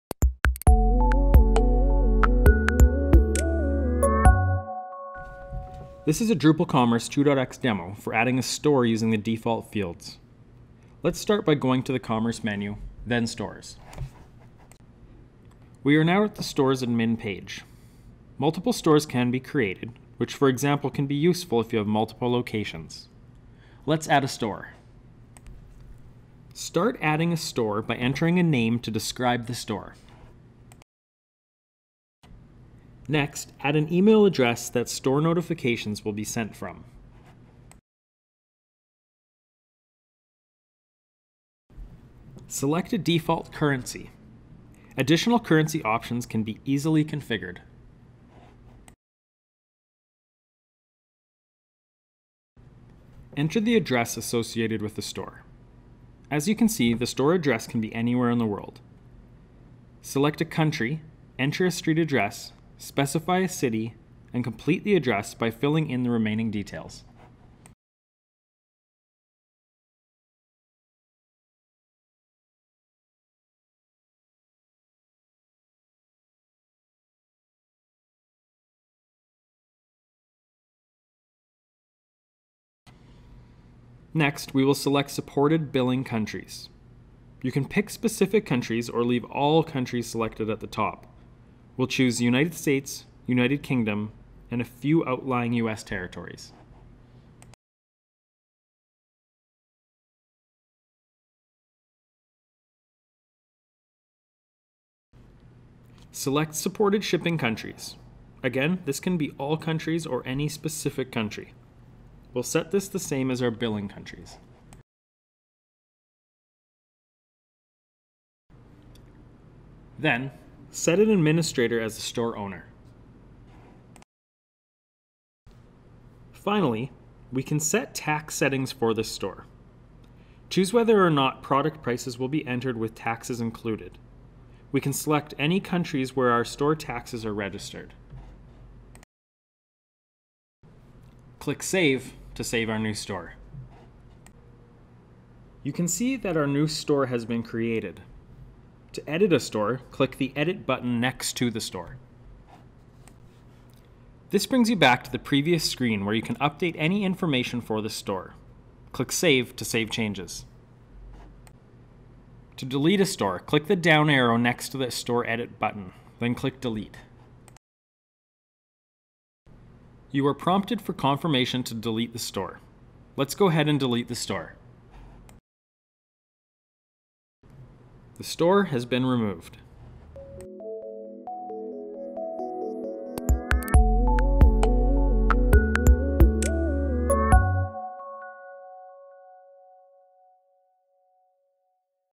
▶️ How To: Add Store Default Fields in Drupal Commerce
Is your company growing, and you're looking to add a store to your Drupal Commerce 2 site?

In this video, we walk through the steps of adding a new store using the default fields. We also show you how to delete a store, just in case!